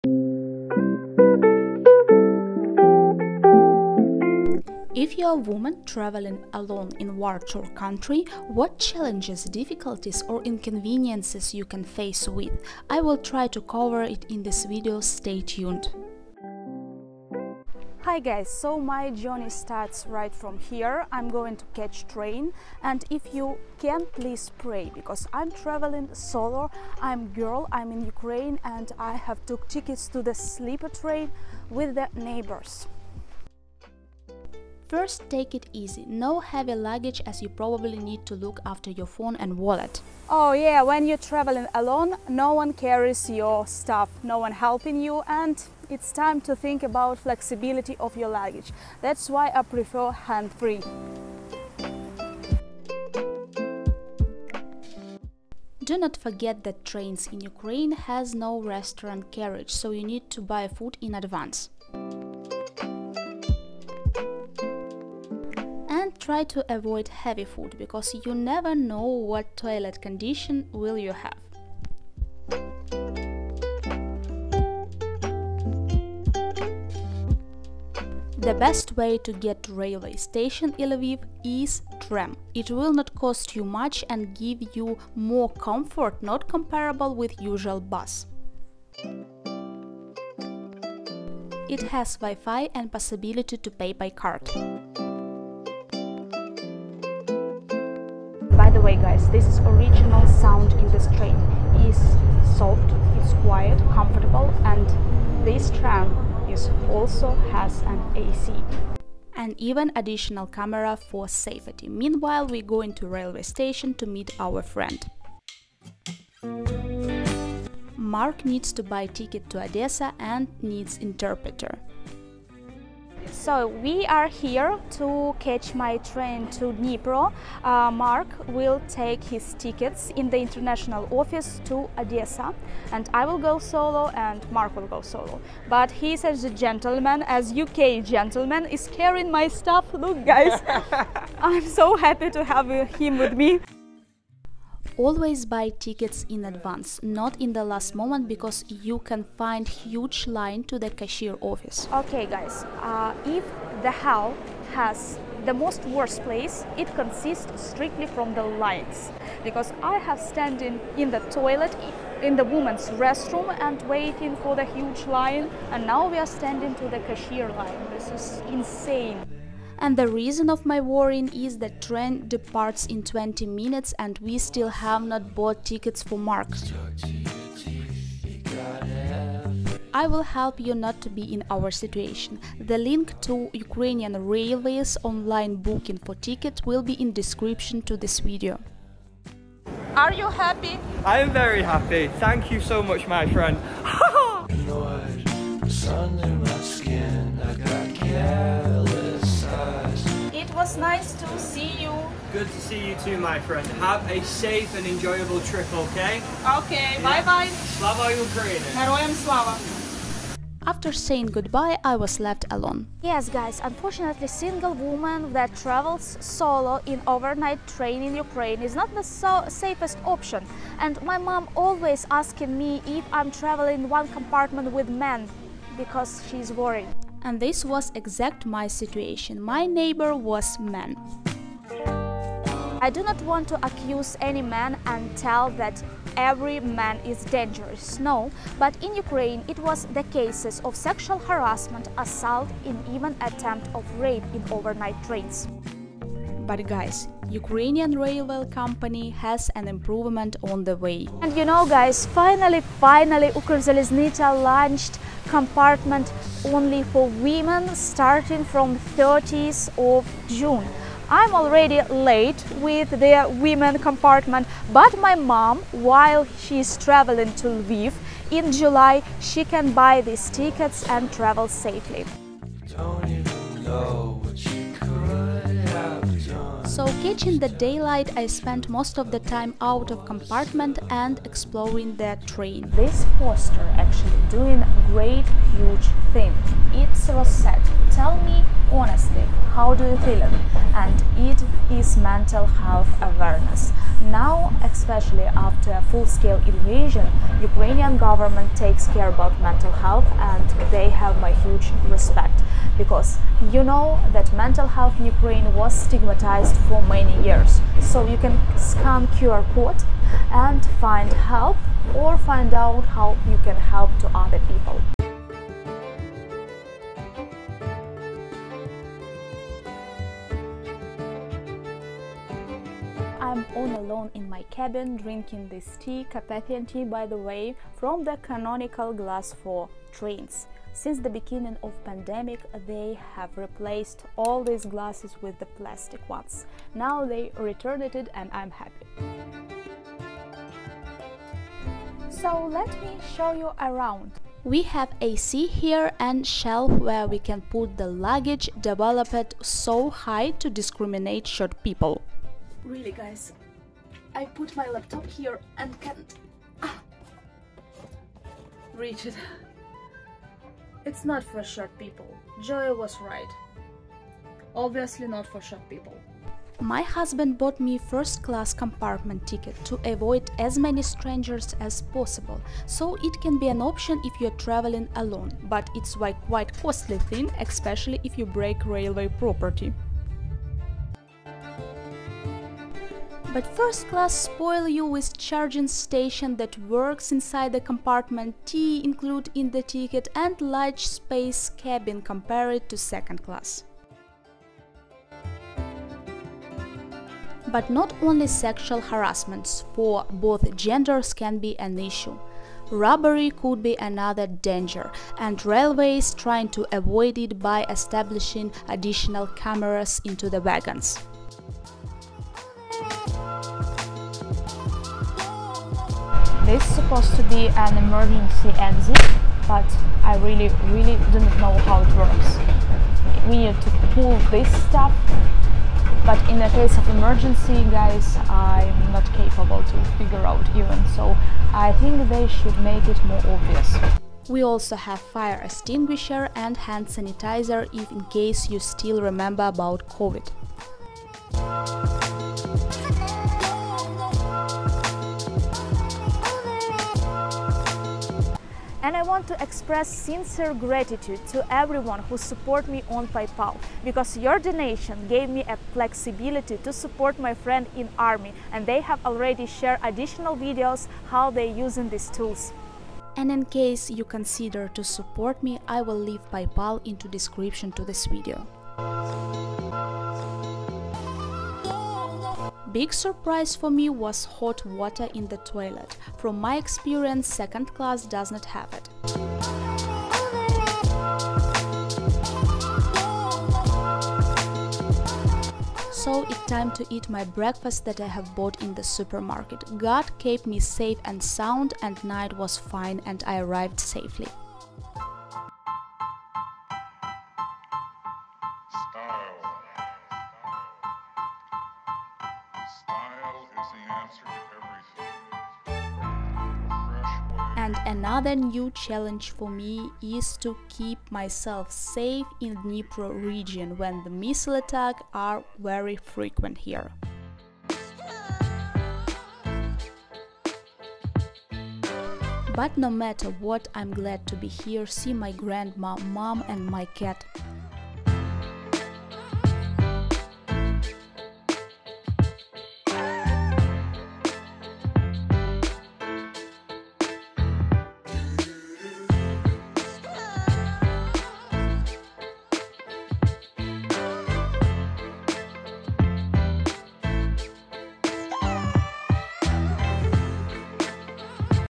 GIRL TRAVELS SOLO ACCROS UKRAINE: FIRST CLASS OVERNIGHT TRAIN FROM LVIV TO DNIPRO
This is my personal story about how traveling by train (first class) from Lviv to the EAST of Ukraine - city Kamenskoye, Dnipro region during the war in Ukraine in 2023.

I took a first-class compartment with one neighbor and spent 16 hours (922 km) on the train; it cost arm and leg - 55 dollars for first class.

Bedsheets, water, napkins, towels, and tea were included in the process. The wagon has AC and hot water.

My hometown Kamenskoye (Dnipro region), is 100 km away from the frontline, and people there are suffering from missile attacks every day.

Please follow me in Twitter to receive fresh daily updates about life of civilian in wartime